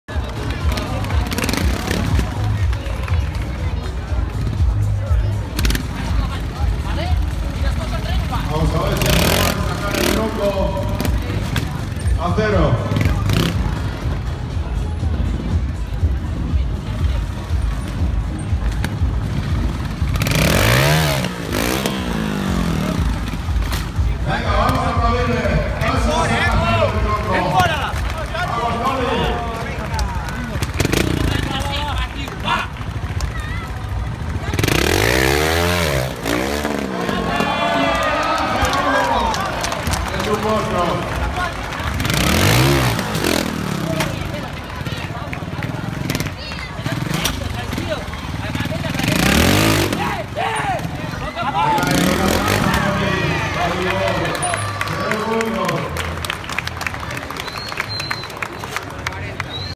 Toni Bou, en zona 12
Toni Bou, en zona 12, Primera Vuelta. Prueba del Campeonato de España de Trial.